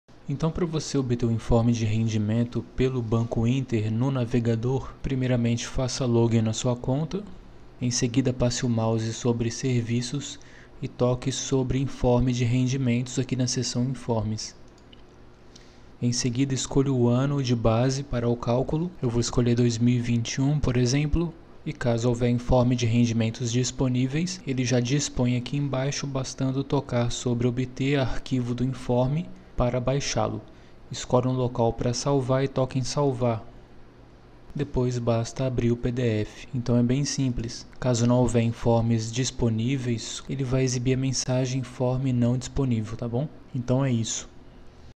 Como Obter Informe De Rendimentos Banco INTER Pelo PC ( ATUALIZADO )
Nesse tutorial vamos aprender como obter o informe de rendimentos da sua conta bancária no Banco Inter pelo Internet Banking no PC.

👉O Tutorial Te Ajudou? Considere Fazer Uma Doação Para Incentivar A Produção De Novos Tutoriais No Canal